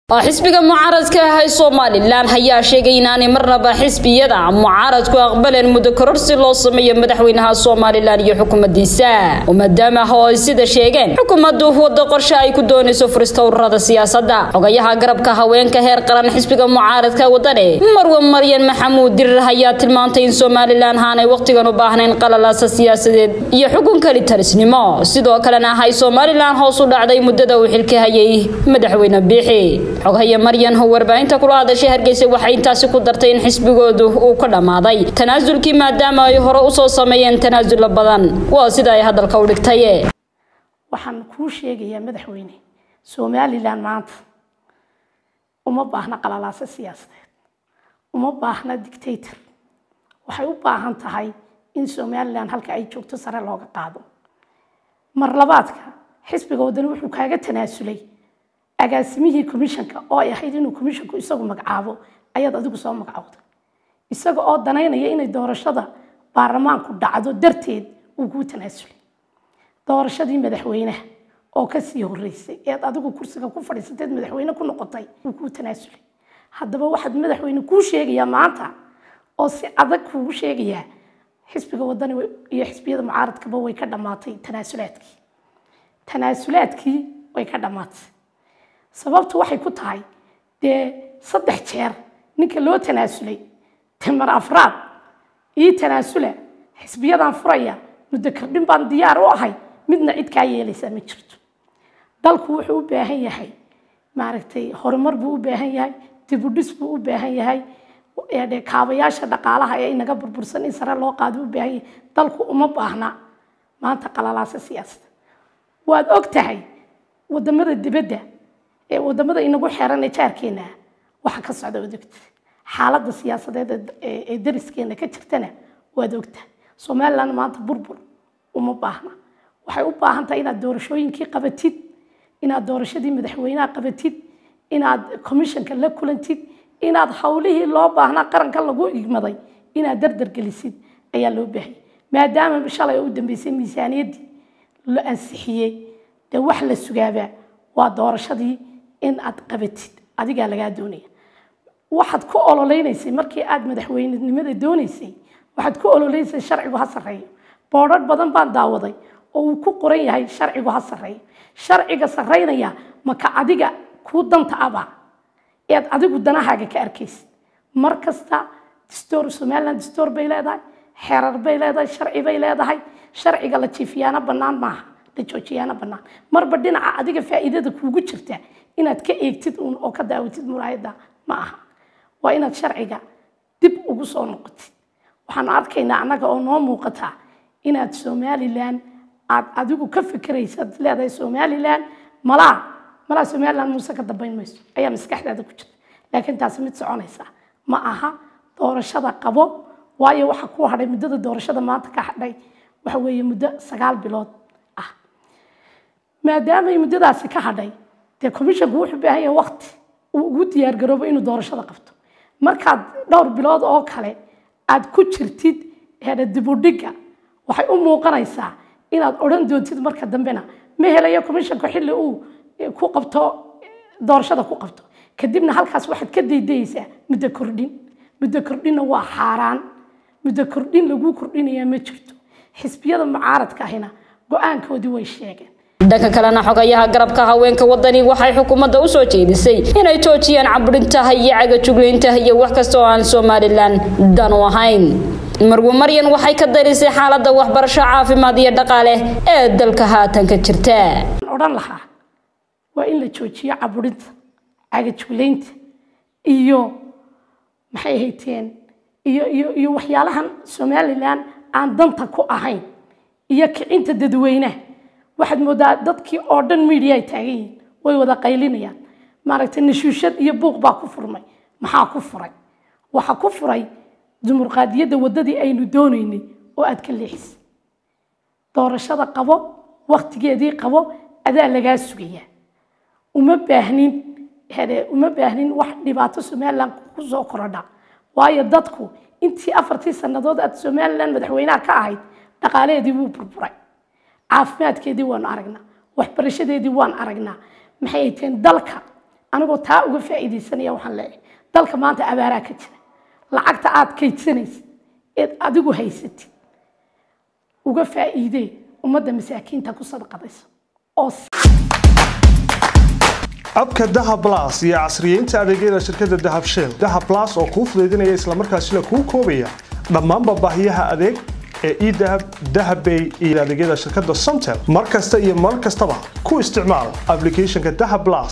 "DR C/RAXMAAN CIRRO DOORASHADII MADAXWEYNAHA AYUU KU TANAASULEY IMIKANA TANASULADKI UU KA DHAMAADEY"
"DR C/RAXMAAN CIRRO DOORASHADII MADAXWEYNAHA AYUU KUU TANAASULEY MADAXWEYNE IMIKANA TANASULAADKII UU KA DHAMAADEY" XOG-HAYAH A GARABKA HAWENKA WADANI MARWO MARYAN MAXAMUD DIRIR